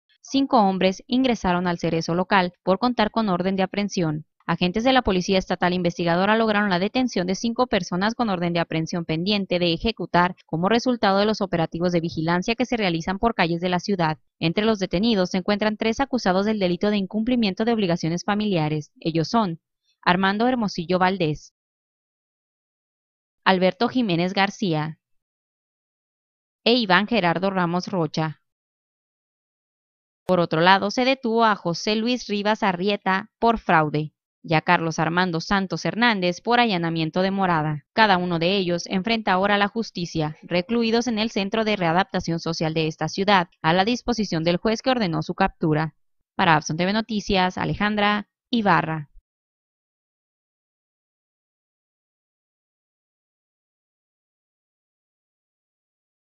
Cinco hombre ingresaron al Cereso local por contar con orden de aprehensión.
Cinco hombre ingresaron al Cereso local por contar con orden de aprehensión.Canal 4 Agua Prieta Sonora